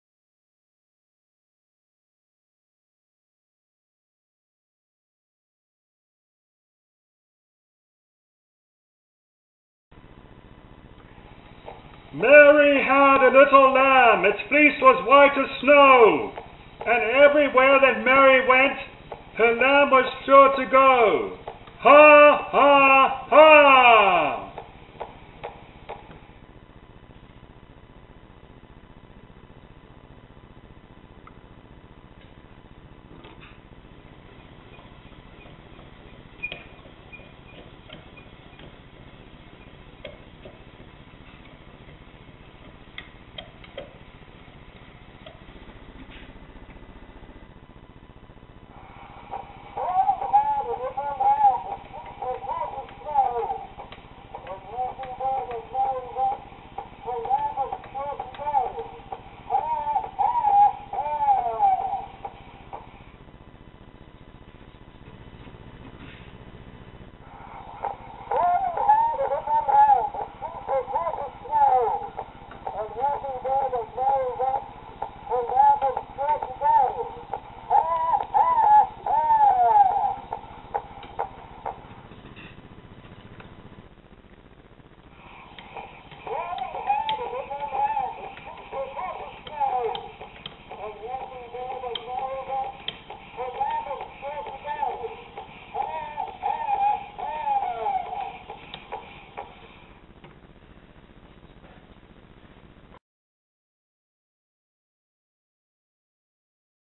THE SCIENTIFIC AMERICAN 1878 TINFOIL PHONOGRAPH MADE IN APRIL 2012 IN SOUTH AFRICA
This was originally a DIY project published on July 20th 1878 from a original American magazine called Scientific American. Together with help from a fellow work colleague in Mamelodi, South Africa and a one year online tuition and coaching from Mr Charles Smith from Connecticut, USA, we got a working model built. This was a realization of a 24 year dream that finally come true!

It was assembled with parts from Connecticut, USA and Mamelodi, Republic of South Africa, so is a joint USA-RSA project! Thank you to everyone who helped! Here are the fruits of your endeavor!!

The tinfoil phonograph was the 2nd machine in the world to record sound but ALSO play it back on the tin foil format! This machine was preceded by Leon Scott de Martinville's phonautograph which recorded sounds only onto smoked paper in the 1850s-1860s and his machine was used as a visual study of sound.

The Scientific American phonograph was released in 1878, the year after the first phonograph was built by Thomas Edison in December 1877. Since 1989 I have been wanting to build a working model and in April 2012 the dream came true! Keep hanging onto your dreams! They WILL come true! Rene Rondeau, thank you for inspiring me more than you will ever know (I watched your participation in the 2008 ARSC Conference over and over!!) AND Mr Charles Smith who had faith in me from day one! Charles, here is our machine!